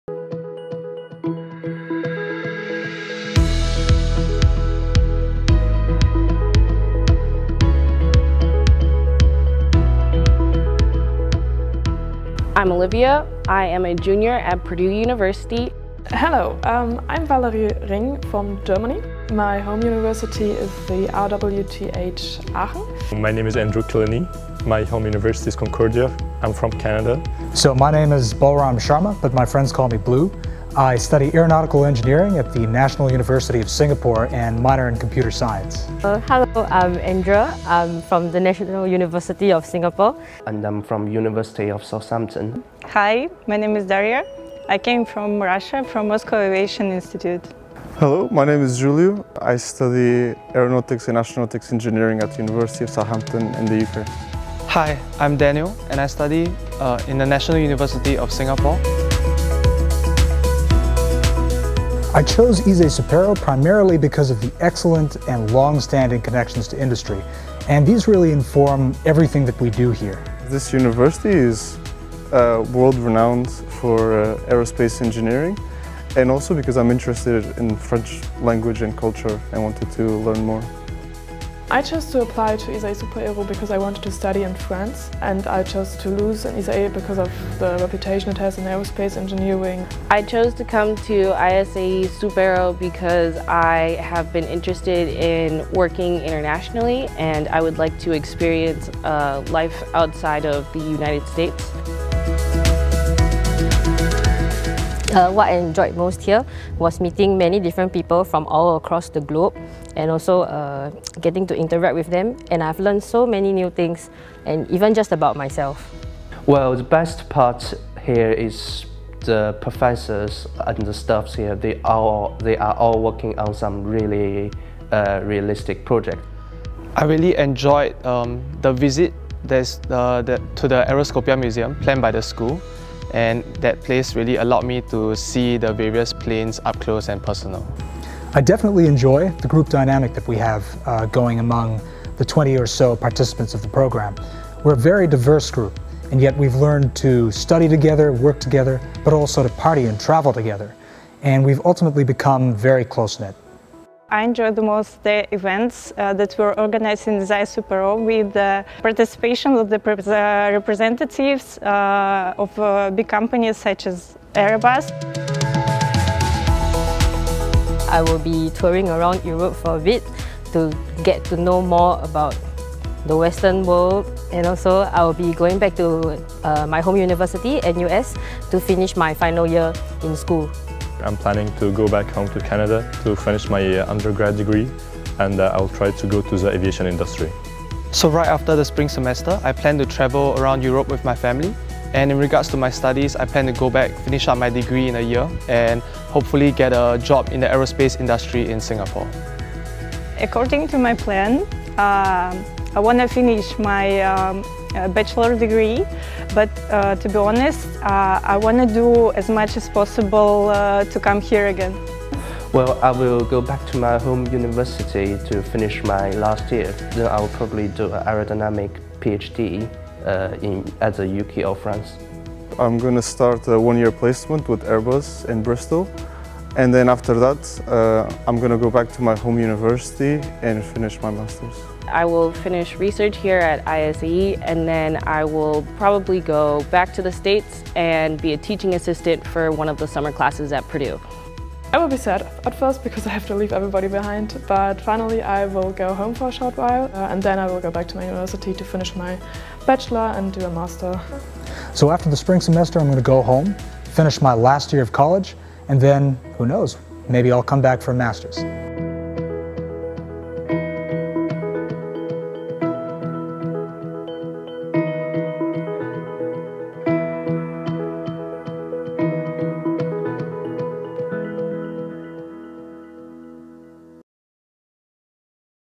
SSAE PROGRAM / Spring Semester in Aeronautical Engineering
The SSAE program is a program for international students .
A full semester in English for junior and senior bachelor students in aeronautical or mechanicalengineering program.